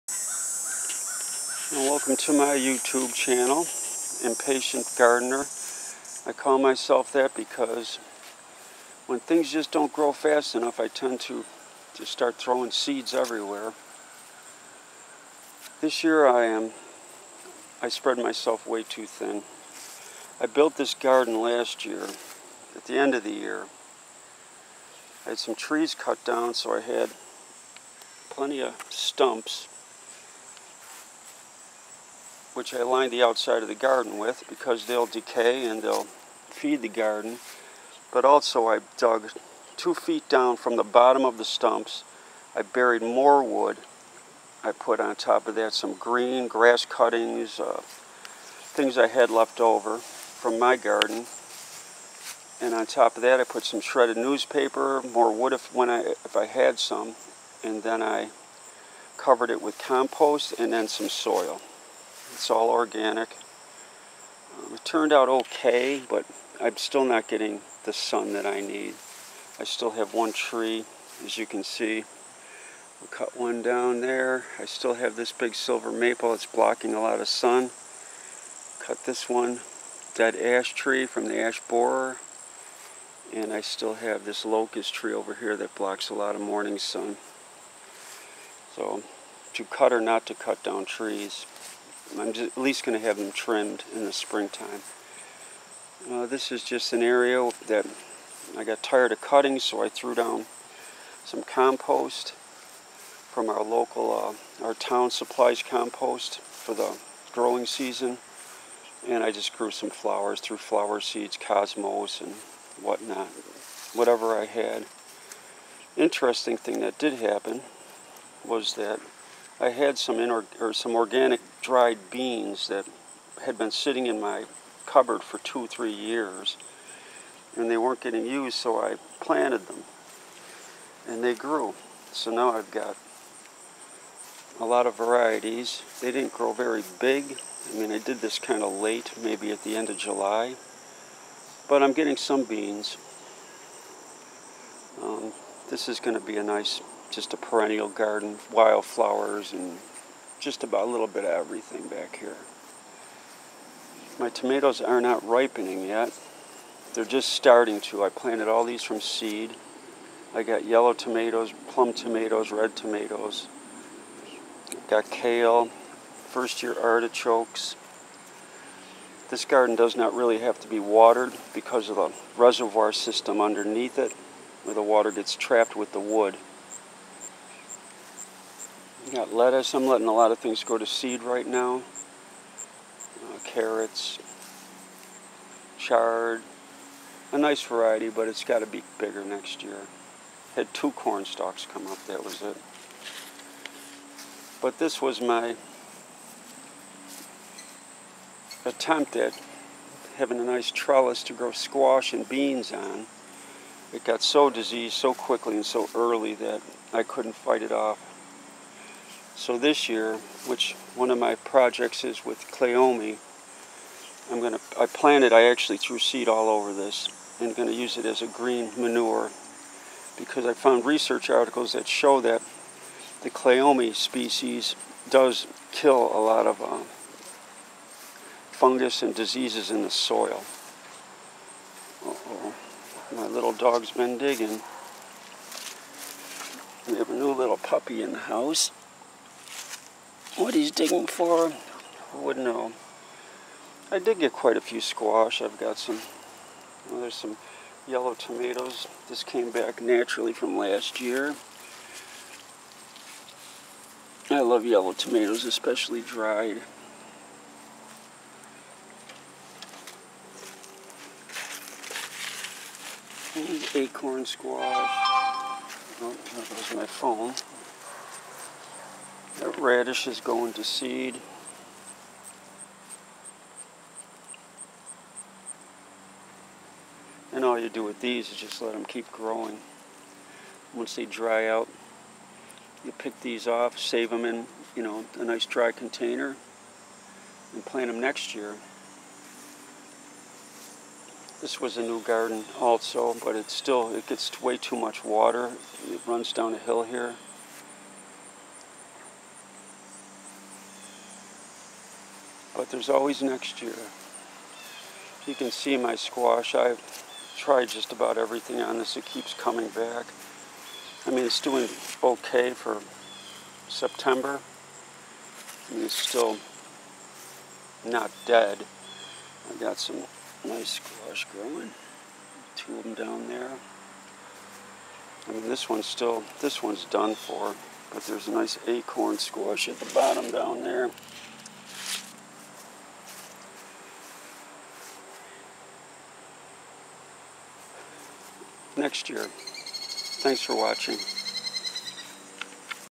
1st year permaculture vegetable garden zone 6 US
I outlined my garden with  sections of stump and large branches,. I then dug 2' down and placed more sections of wood, and on top of that, grass and cuttings I had from last year, on top of that shredded paper, then compost, then soil. It needed very little watering this season, and I only sprayed the plants on very hot days we had. I think this first season shows great promise.